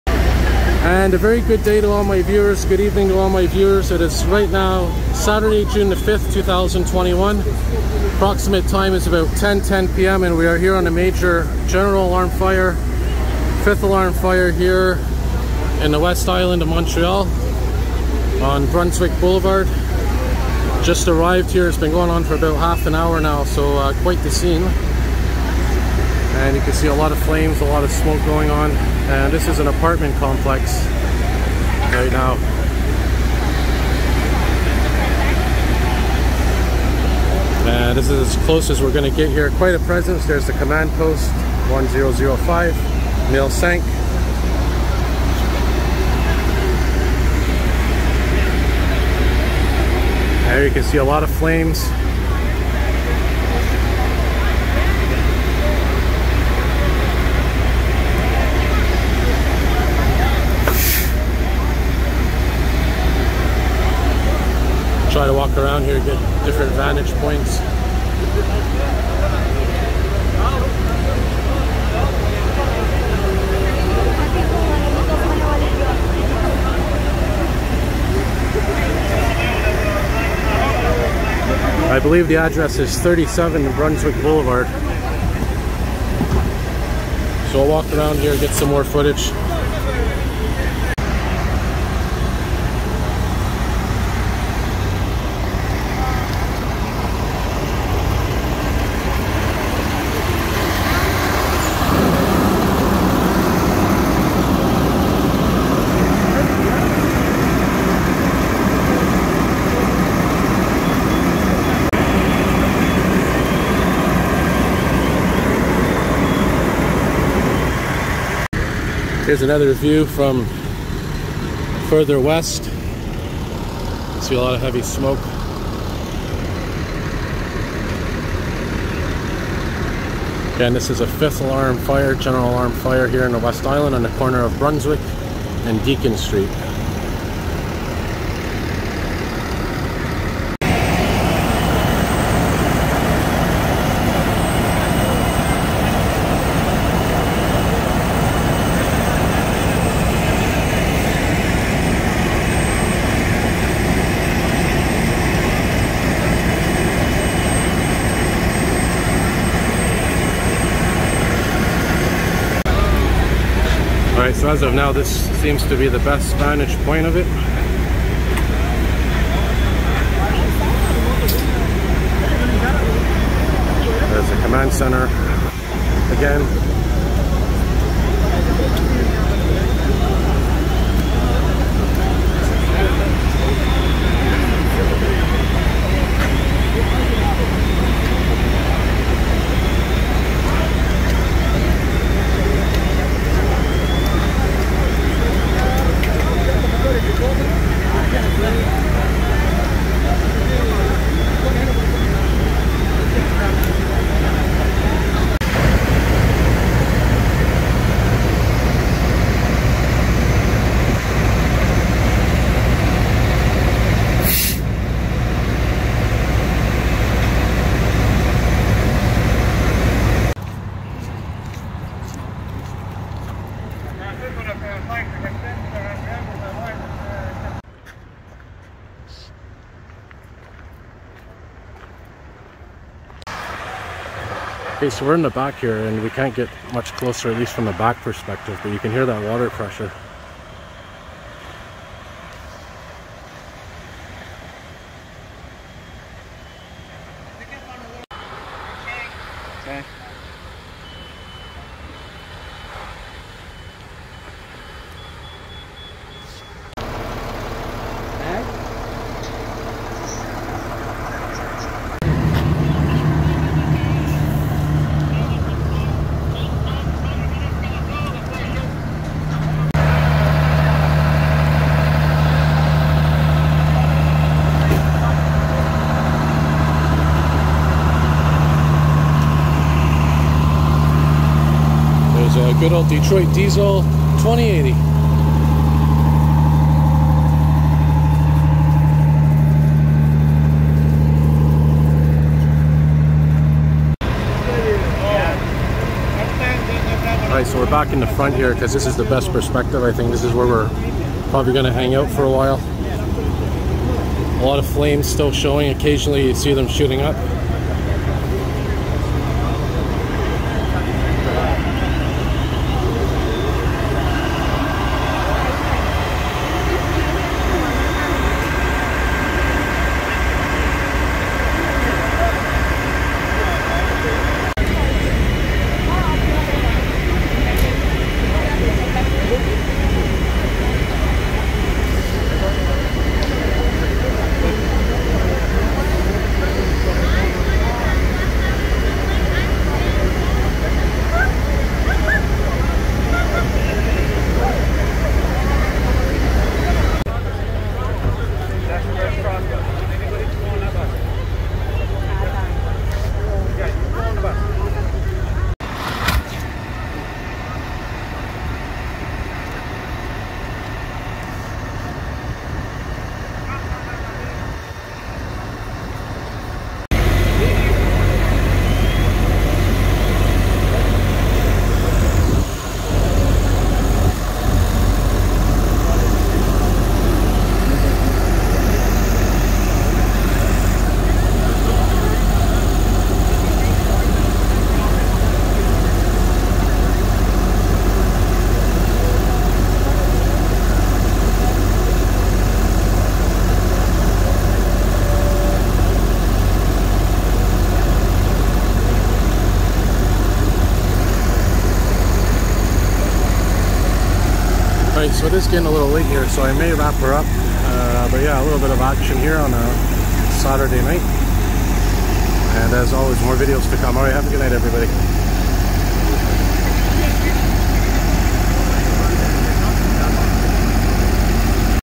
🔥 5th alarm fire in a large apartment complex on Brunswick Street in the West Island of Montreal
On the evening of Saturday, June 5, 2021, a general alarm / 5th alarm fire broke out in an apartment complex at 37 Brunswick Street in the west Island of Montreal.